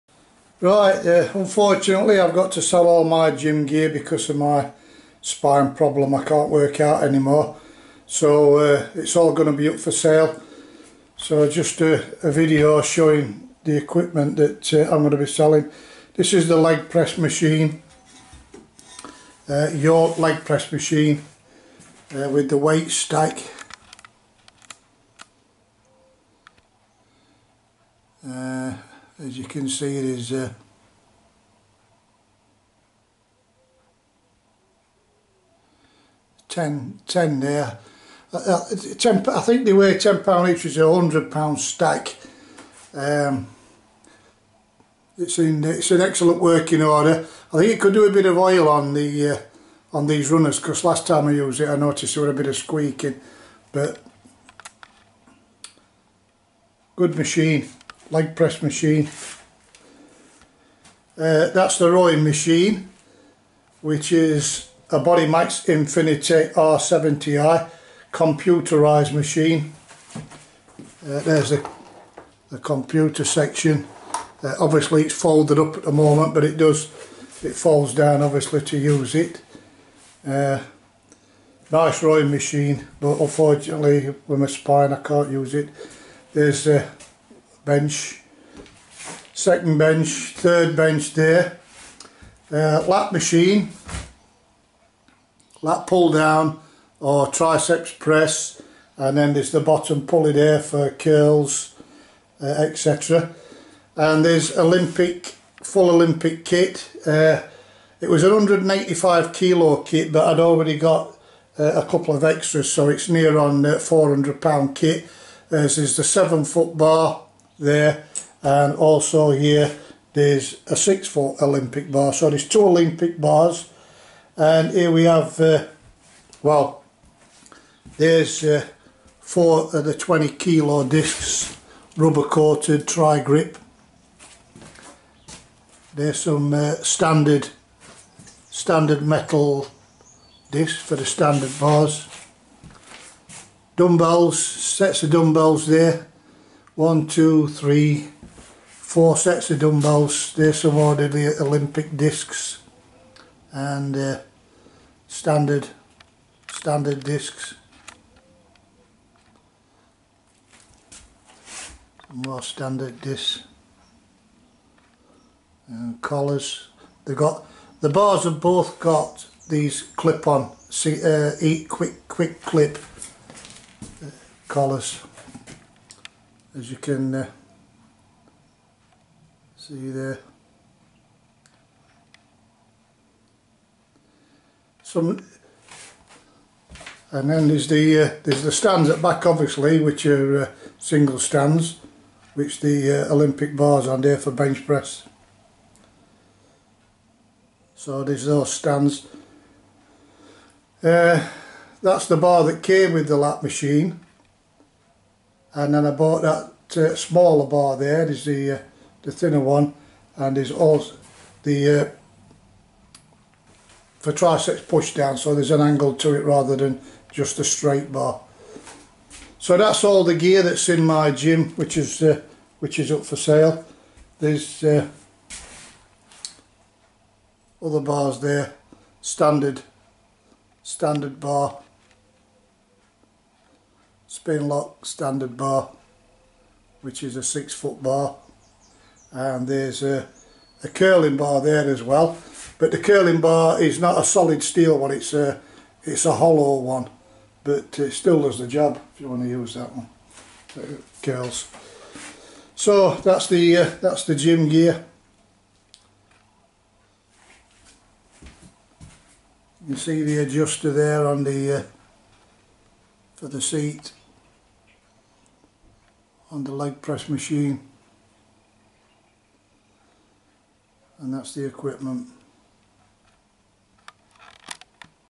Gym equipment for sale
No longer able to workout due to degenerative spine injury so my equipment is up for sale.